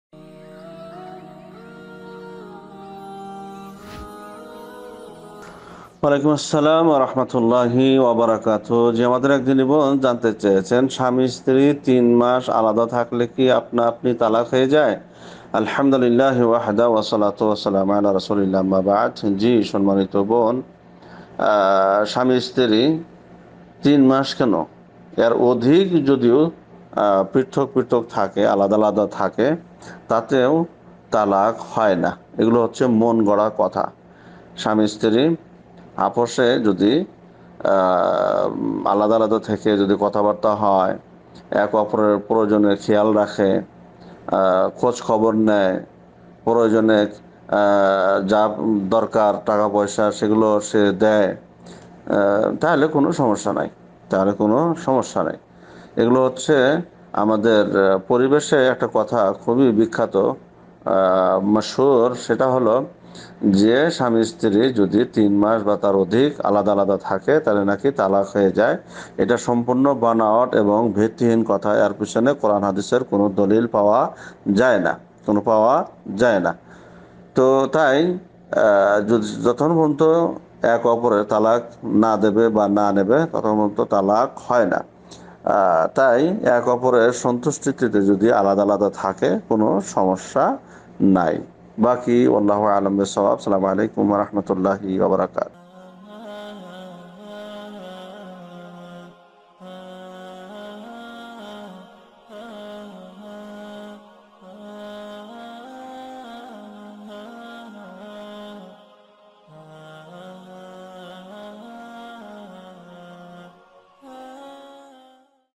স্বামী স্ত্রী ৩ মাস আলাদা থাকলে কি তালাক হয়ে যায়│শায়েখ মাহবুবুর রহমান বুখারী মাদানি bangla waz
Present by.
Al Huda Wan Noor Foundation
প্রশ্নঃ- স্বামী স্ত্রী ৩ মাস আলাদা থাকলে কি তালাক হয়ে যায়
উত্তর দিয়েছেনঃ- শায়েখ মাহবুবুর রহমান বুখারী মাদানি